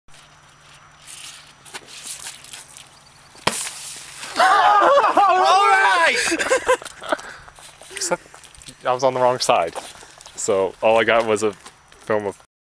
Diet Coke and Mentos Rocket Success SV
Our first good success, this one shot across the street after hitting the little wash.